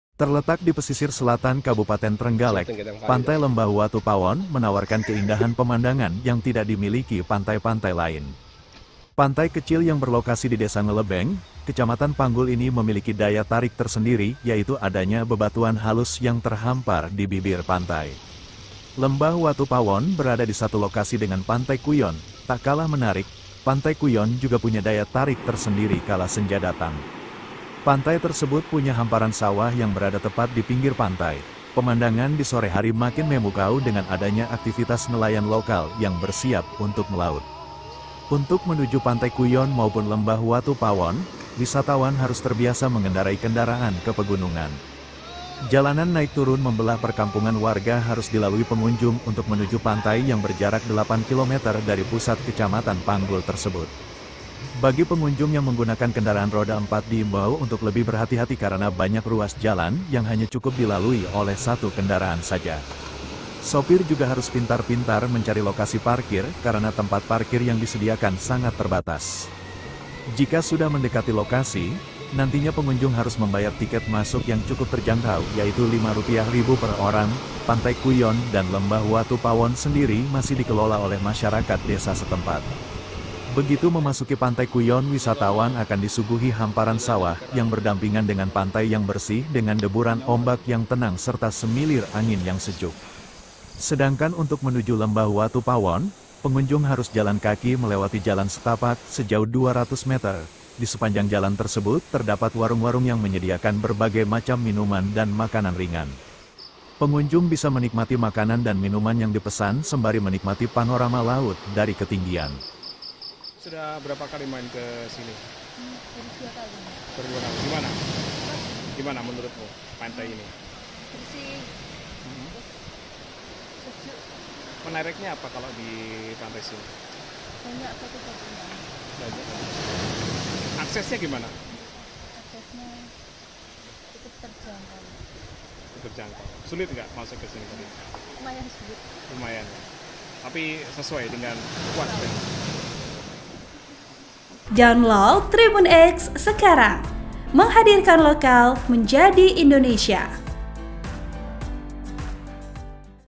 Pesona Lembah Watu Pawon, Pantai dengan Sunset Memukau di Pesisir Selatan Trenggalek
video & reporter : sofyan arif candra / zaq

Terletak di pesisir selatan Kabupaten Trenggalek, Pantai Lembah Watu Pawon menawarkan keindahan pemandangan yang tidak dimiliki pantai-pantai lain.
Pantai kecil yang berlokasi di Desa Nglebeng, Kecamatan Panggul ini memiliki daya tarik tersendiri yaitu adanya bebatuan halus yang terhampar di bibir pantai.
Lembah Watu Pawon berada di satu lokasi dengan Pantai Kuyon. Tak kalah menarik, Pantai Kuyon juga punya daya tarik tersendiri kala senja datang.
Pantai tersebut punya hamparan sawah yang berada tepat di pinggir pantai. Pemandangan di sore hari makin memukau dengan adanya aktivitas nelayan lokal yang bersiap untuk melaut.
Untuk menuju Pantai Kuyon maupun Lembah Watu Pawon, wisatawan harus terbiasa mengendarai kendaraan ke pegunungan.
Jalanan naik turun membelah perkampungan warga harus dilalui pengunjung untuk menuju pantai yang berjarak 8 kilometer dari pusat Kecamatan Panggul tersebut.
Bagi pengunjung yang menggunakan kendaraan roda 4 diimbau untuk lebih berhati-hati karena banyak ruas jalan yang hanya cukup dilalui oleh satu kendaraan saja.
Sopir juga harus pintar-pintar mencari lokasi parkir karena tempat parkir yang disediakan sangat terbatas.
Jika sudah mendekati lokasi, nantinya pengunjung harus membayar tiket masuk yang cukup terjangkau yaitu Rp 5 ribu per orang. Pantai Kuyon dan Lembah Watu Pawon sendiri masih dikelola oleh masyarakat desa setempat.
Begitu memasuki Pantai Kuyon wisatawan akan disuguhi hamparan sawah yang berdampingan dengan pantai yang bersih dengan deburan ombak yang tenang serta semilir angin yang sejuk.
Sedangkan untuk menuju Lembah Watu Pawon, pengunjung harus jalan kaki melewati jalan setapak sejauh 200 meter. Di sepanjang jalan tersebut terdapat warung-warung yang menyediakan berbagai macam minuman dan makanan ringan.
Pengunjung bisa menikmati makanan dan minuman yang di pesan sembari menikmati panorama laut dari ketinggian.
Seorang pengunjung Lembah Watu Pawon, Yenyen mengaku sudah dua kali mengunjungi pantai tersebut.
Menurut Yeyen, akses menuju Lembah Watu Pawon memang relatif sulit namun itu semua terbayarkan dengan panorama yang memanjakan mata.
Senada, pengunjung lainnya, Clara menilai Lembah Watu Pawon punya keunikan tersendiri dengan adanya bebatuan yang bulat-bulat layaknya batu sungai.
Di tempat terpisah Ketua Kelompok Sadar Wisata (Pokdarwis) Suka Maju, Desa Nglebeng, Kecamatan Panggul, Juni, mengatakan kunjungan wisatawan ke Lembah Watu Pawon dan Pantai Kuyon mengalami tren kenaikan pada akhir pekan dari hari - hari besar nasional.
Pada hari - hari biasa, pengunjung Pantai Kuyon dan Lembah Watu Pawon hanya di angka 200 orang per hari namun saat akhir pekan mencapai 600 orang.
Pihak pengelola telah menyediakan 4 toilet portable di Pantai Kuyon dan Lembah Watu Pawon. Selain itu pengelola juga menyediakan aula jika ada instansi atau perusahaan, dan pengunjung yang ingin mengadakan rapat dan pertemuan lainnya di Pantai Kuyon.
Sedangkan dari sisi fasilitas, pengelola berupa untuk menyediakan musala di pantai serta memperbaiki jalan untuk mempermudah akses wisawatan terutama yang menggunakan kendaraan roda 4.